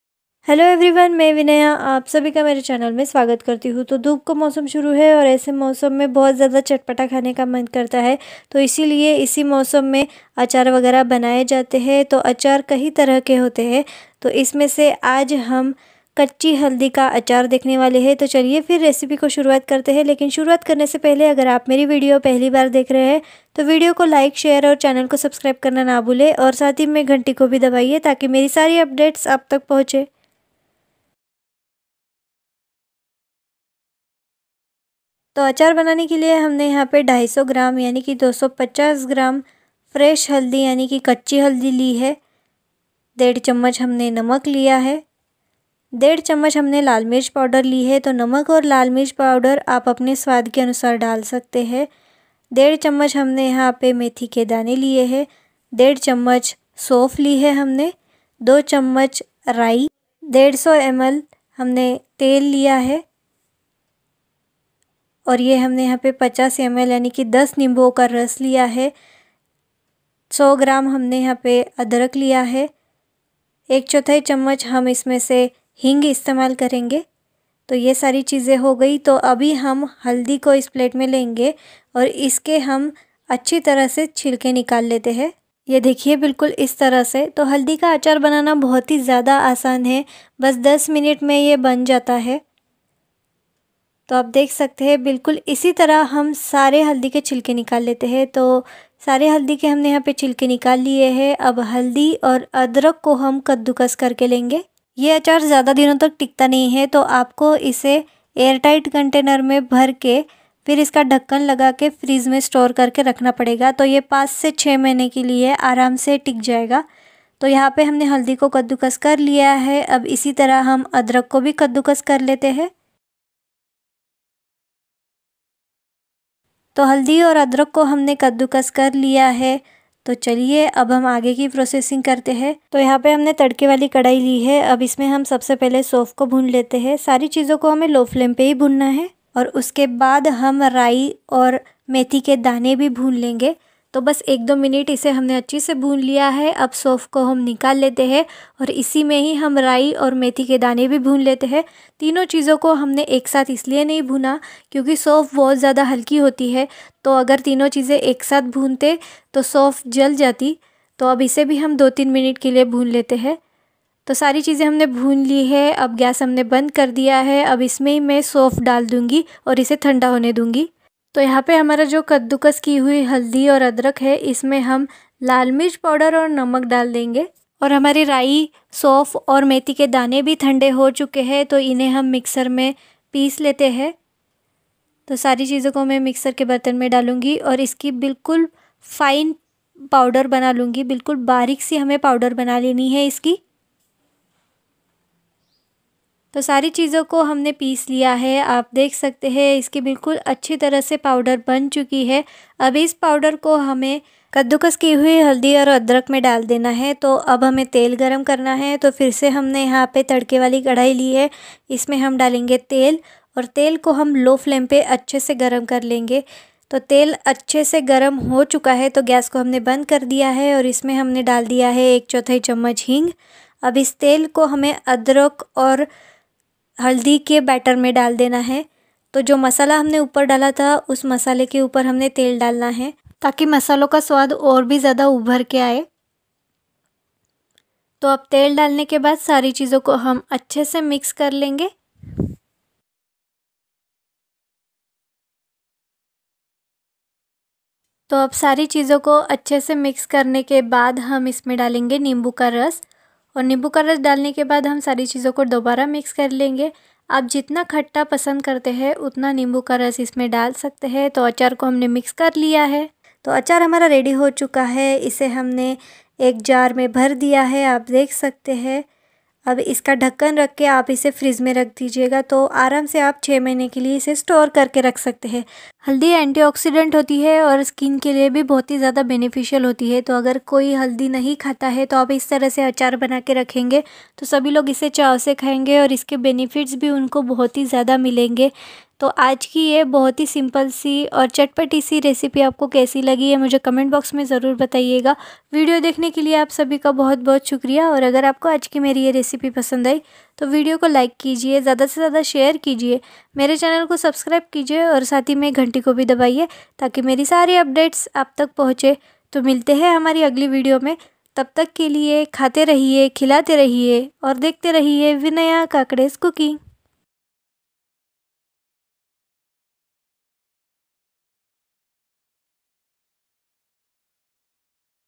Fresh Turmeric Pickle-Pickle-कच्ची हल्दी का अचार-हल्दीका अचार-अचार- ओल्या हळदीचे लोणचे-हळदीचे लोणचे
*Ingredients-

250 g fresh turmeric/कच्ची हल्दी
100 g ginger/अद्रक
150 ml oil/तेल
1.5 tbsp salt/नमक
1.5 tbsp red chilli powder/लाल मिरची पावडर
1.5 tbsp dry fenugreek seeds/मेथी दाणे
2 tbsp mustard seeds/राई
1.5 tbsp fennel seeds/सौफ
50 ml/10 lemon juice/निंबु का रस
1/4 tbsp asafetida/हिंग

video references-